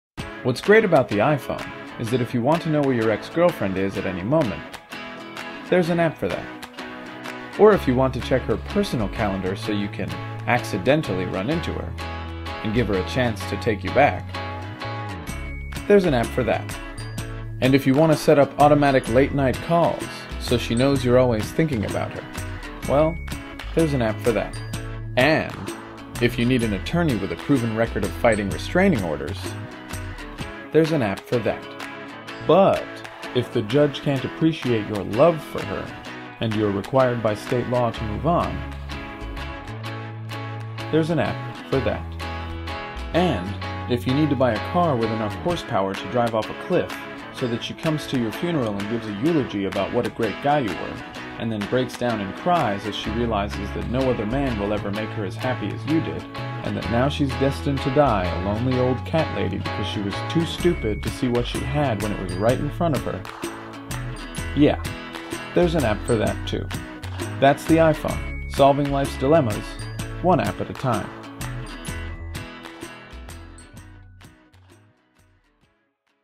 LEAKED: New iPhone Commercial
There really is an app for almost anything, even when there shouldn't be.